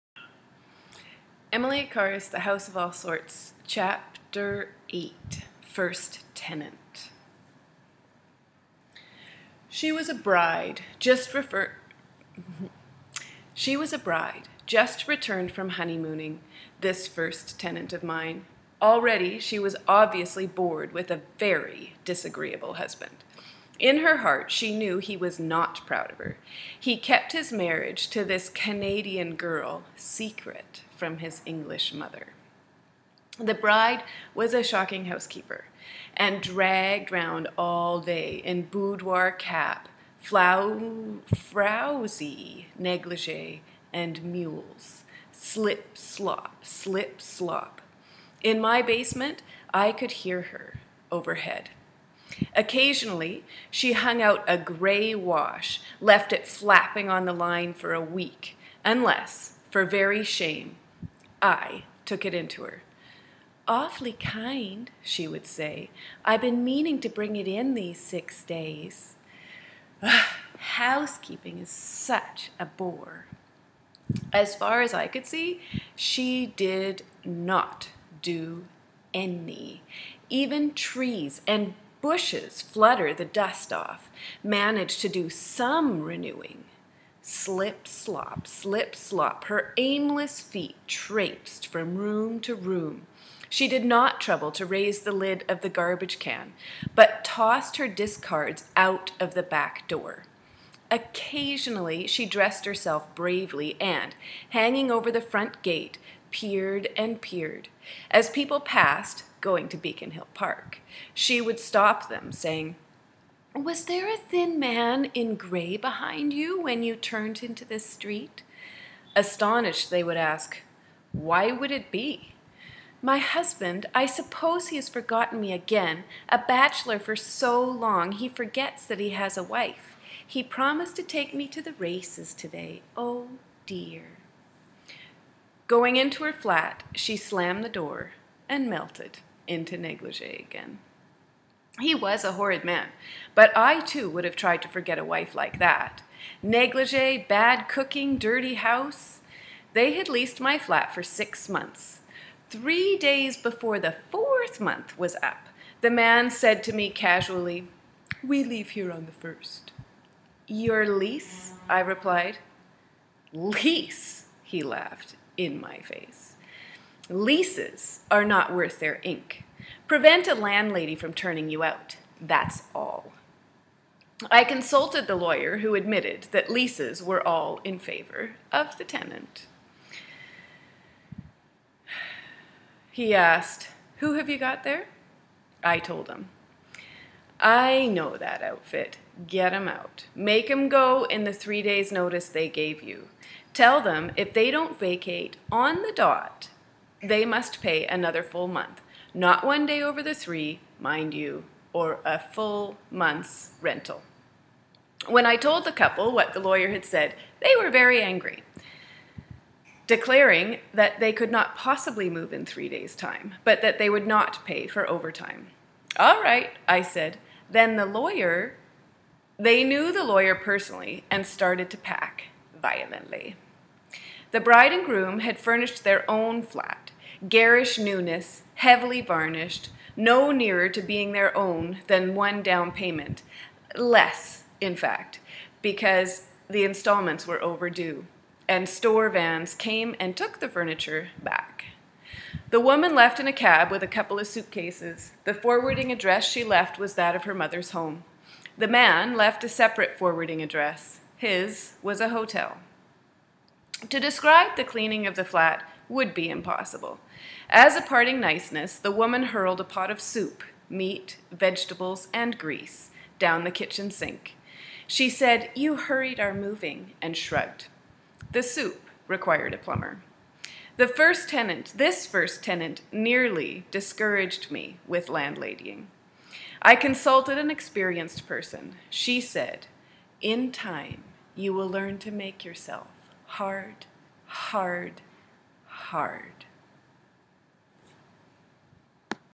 Emily Carr's The House of All Sorts, Chapter 8: First Tenant
Emily is much stronger in this chapter than the last. Learning to live with it, finding her way. Go Emily!